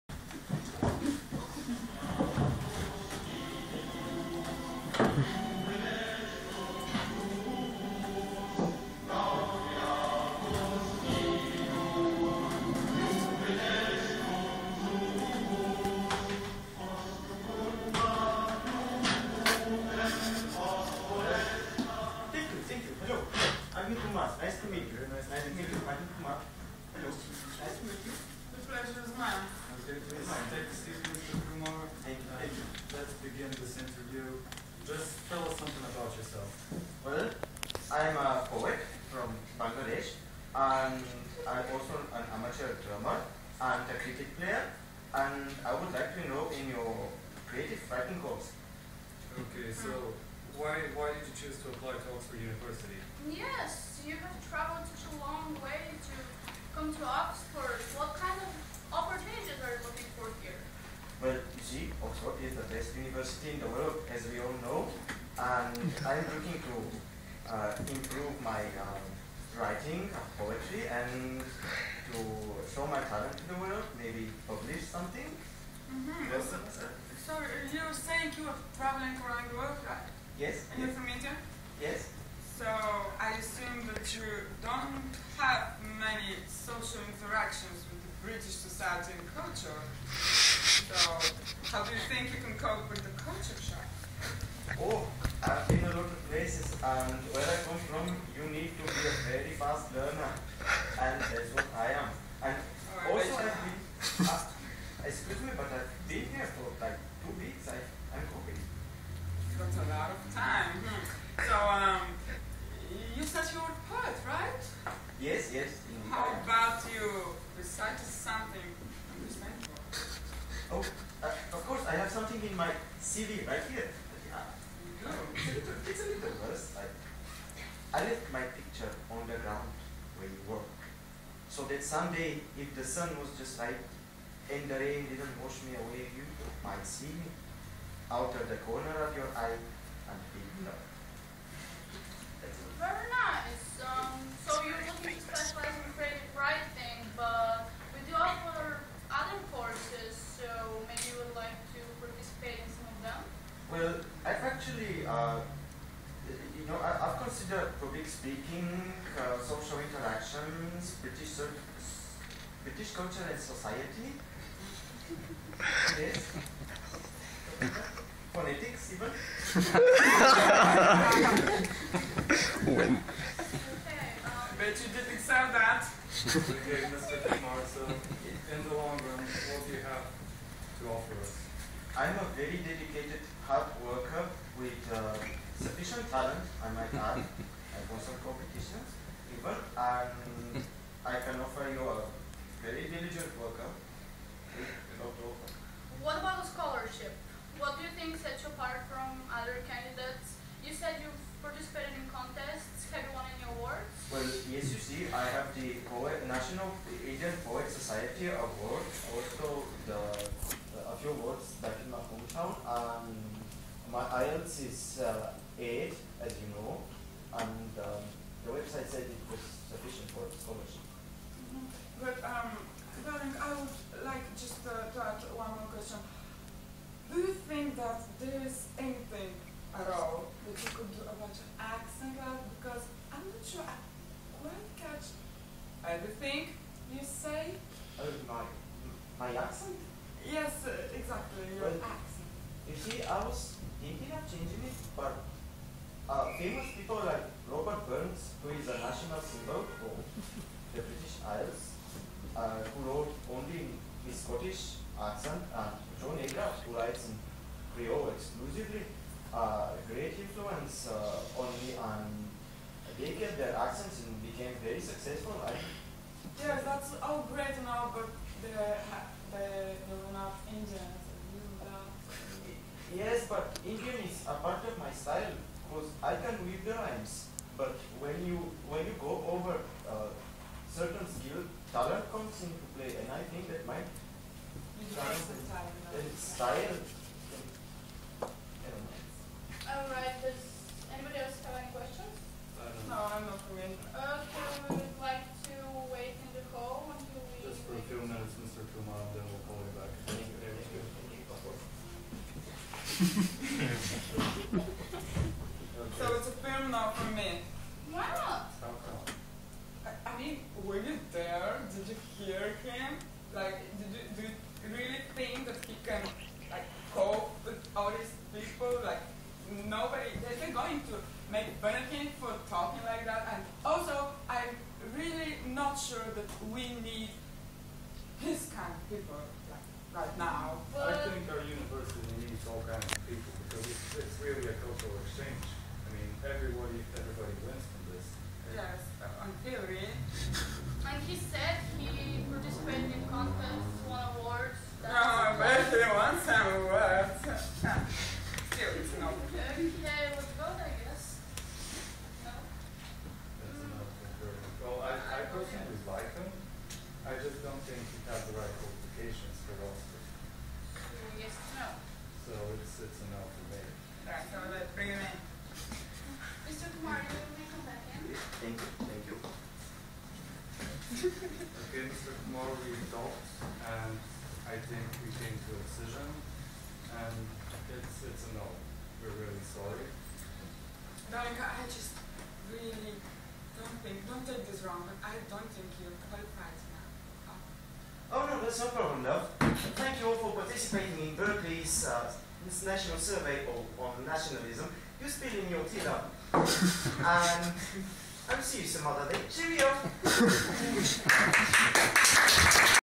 "Academic interview"
Based on Listen Mr Oxford Don by John Agard. Performed by Pavel Valkanov, Toni Pashova, Ilinka Danova, Dimitar Chaushev at The Final Show, English for Social Interaction elective, Sofia University.